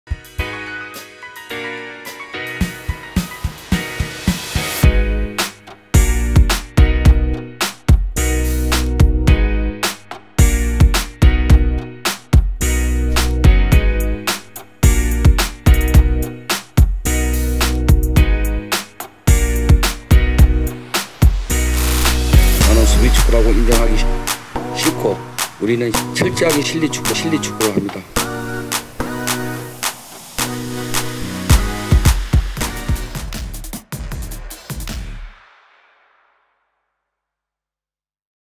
박항서 감독 응원댓글 달고 싸인 유니폼 받자!! [GOAL 이벤트]
박항서 매직으로 아시안컵 8강에 진출한 베트남 국가대표 팀!!
GOAL TV 구독하고 박항서 감독 응원댓글을 남겨주시면
박항서 감독 친필 싸인 베트남 유니폼을 드립니다~
이벤트 추첨은 아시안컵 종료 후 진행됩니다!